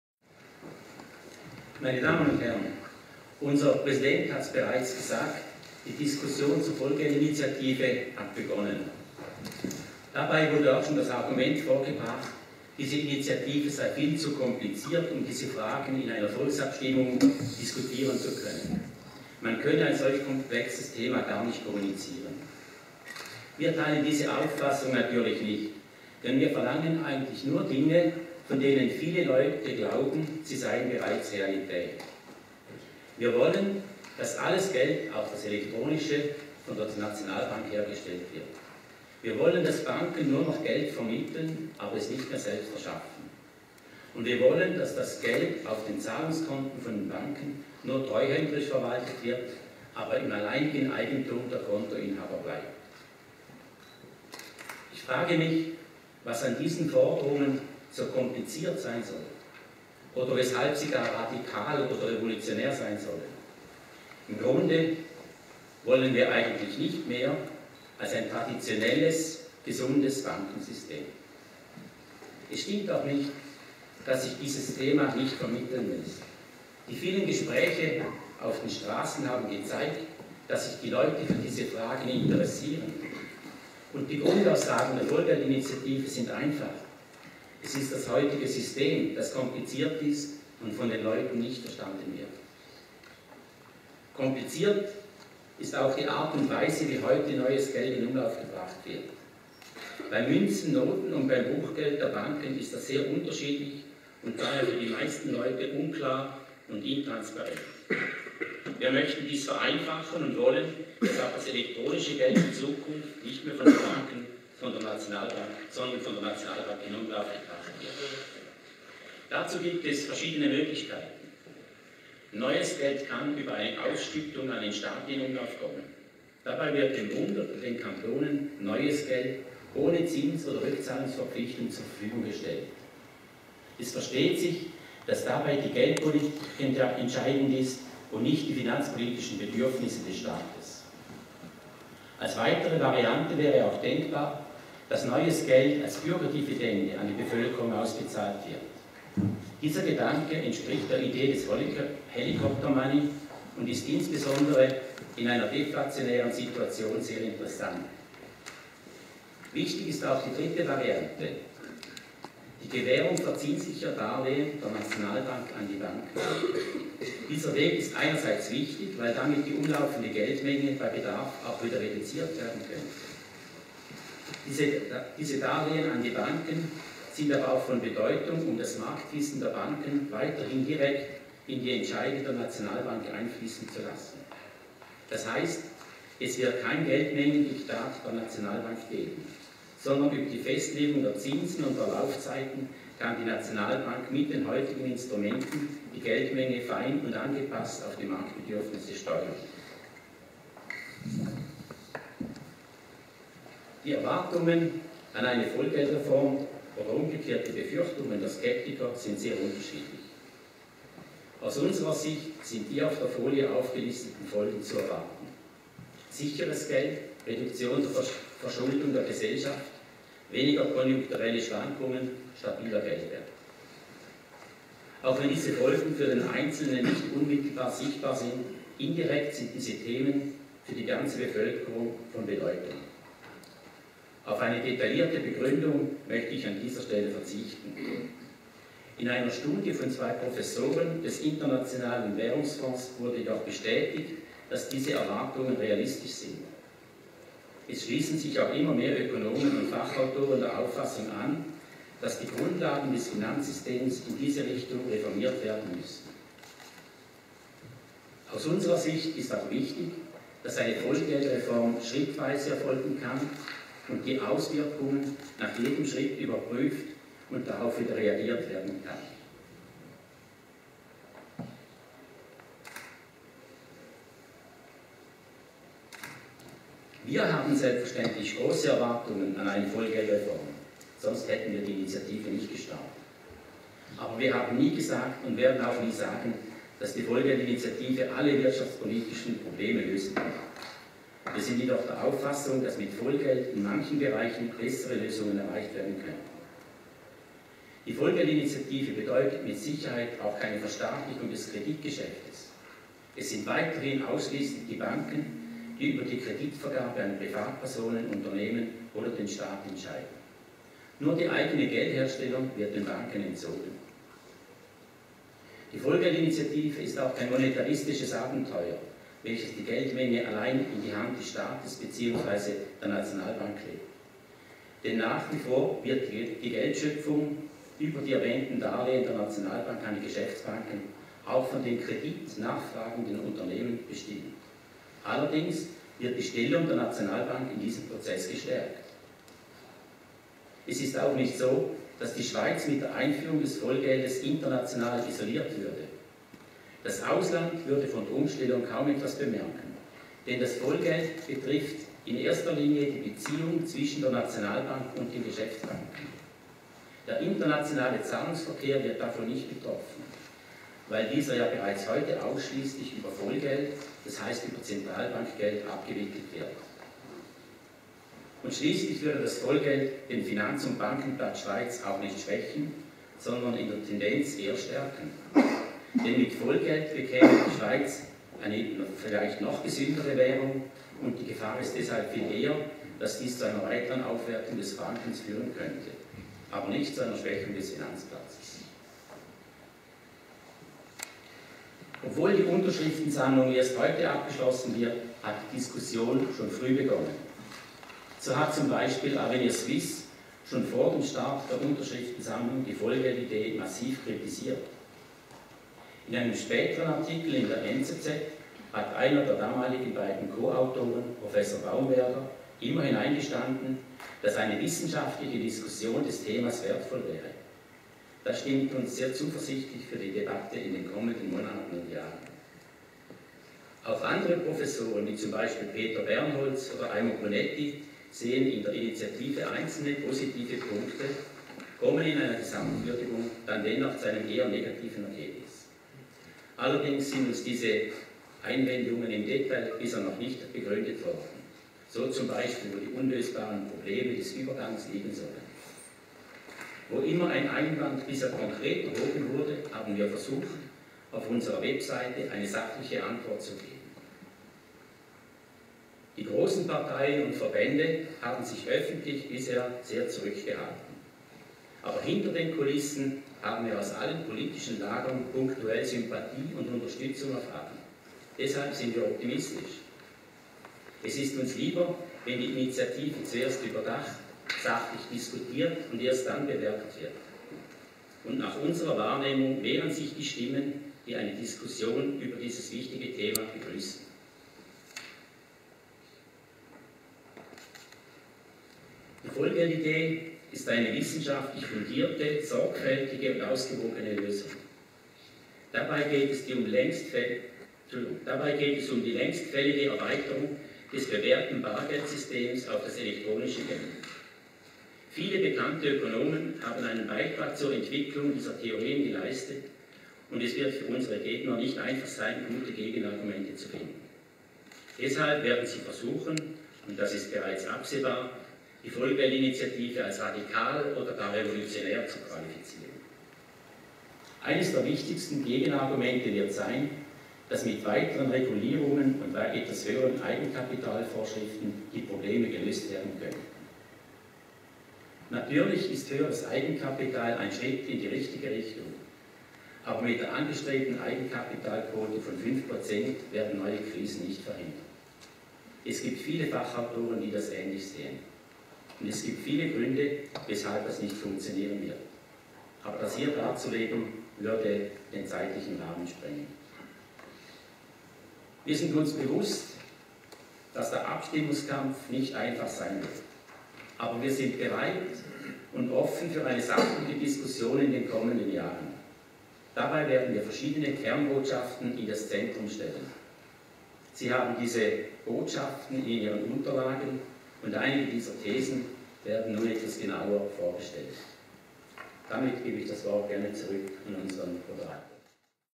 Dr. oec. Reinhold Harringer, Vollgeld-Pressekonferenz 1.12.2015
Pressekonferenz zur Einreichung der
VOLLGELD - INITIATIVE vom 1.12.15
im Medienzentrum des Bundes, Bern.